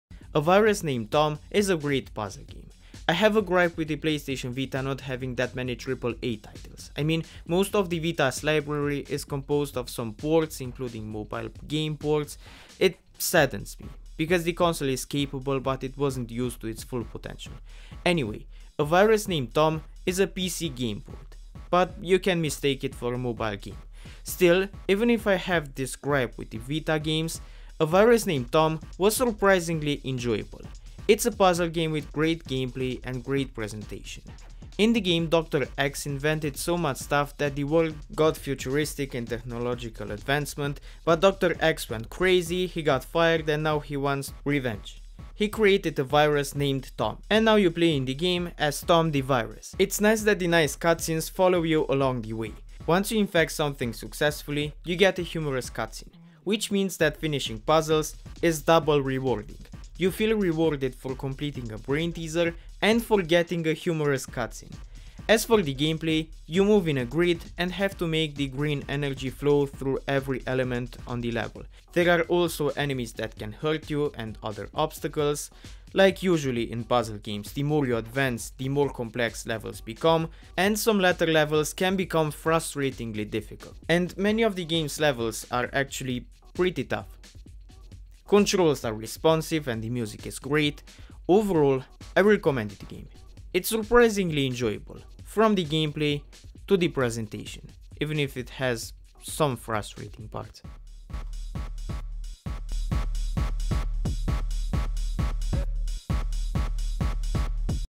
A virus named TOM PS Vita Review
A virus named Tom is a great puzzle game. I have a gripe with the Playstation Vita not having that many triple A titles. I mean most of the Vita’s library is composed of  some ports, including mobile game ports.  It saddens me, because the console is capable, but it wasn’t used to it’s full potential.
 Anyway, A virus named Tom is a PC game port, but you can mistake it for a mobile game. Still, even if I have this gripe with the Vita games, A Virus named Tom was surprisingly enjoyable. It’s a puzzle game with great gameplay and great presentation. In the game Dr. X invented so much stuff that the world got futuristic in tehnological advancement. But Dr. X went crazy, he got fired and now he wants revenge. He created a Virus named Tom. And now you play the game as Tom the virus.
 It’s nice that the nice cutcenes follow you along the way. Once you infect something successfully you get a humorous cutscene. Which means that finishing puzzles is double rewarding. You feel rewarded for completing a brain teaser, and for getting a humorous cutscene.
 As for the gameplay, you move in a grid and have to make the green energy flow through every element on the level. There are also enemies that can hurt you and other obstacles. Like usually in puzzles games, the more you advance, the more complex levels become. Tho some latter levels can become frustratingly difficult.
 Controls are responsive and the music is great.
 Overall, I recommend you the game. It’s surprinzly enjoyable, from the gameplay, to the presentation.